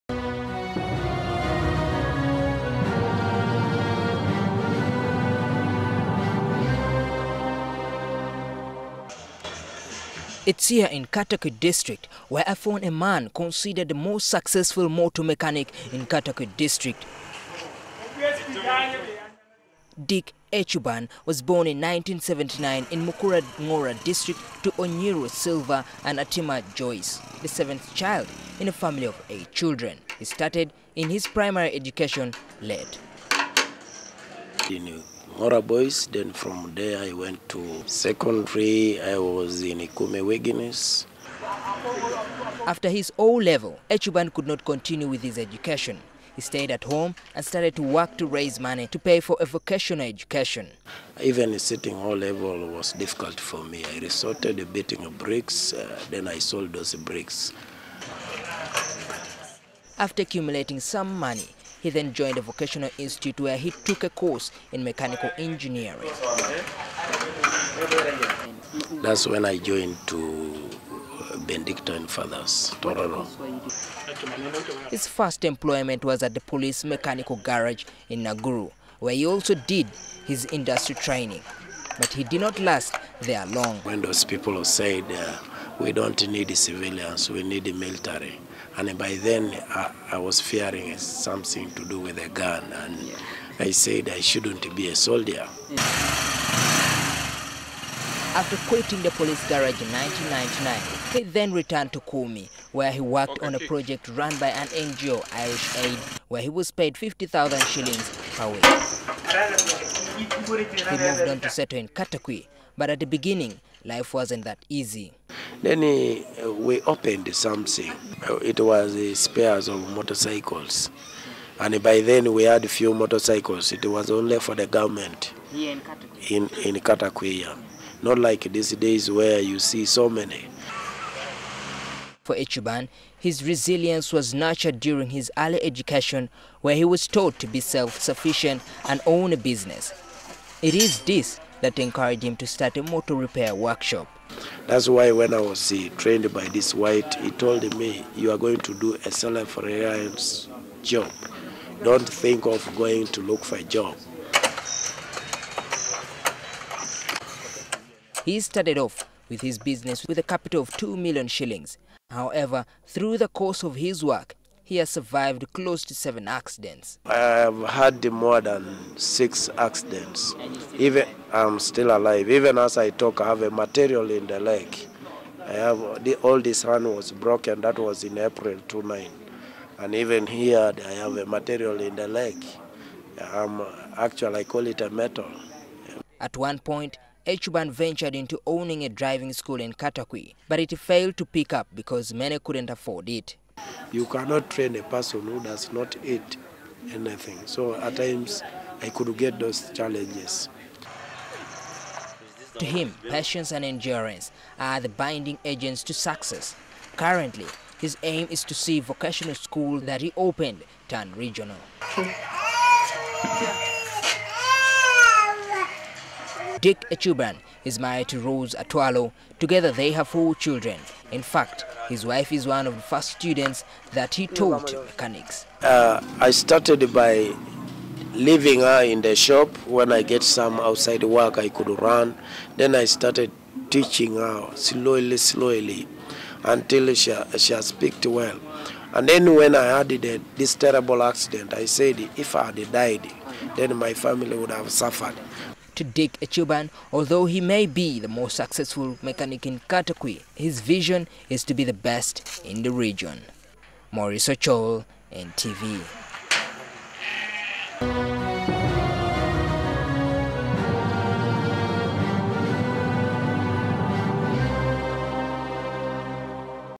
Profile: Eng. Ecuban
He is the most successful Mechanical Engineer in Katakwi  district and owns a Vocational Training Institute, yet Dick Ecuban's life was not a mirror of success years earlier. He  could not  continue with his A 'level studies and joined a vocational institute not realizing that this would be his breakthrough in life.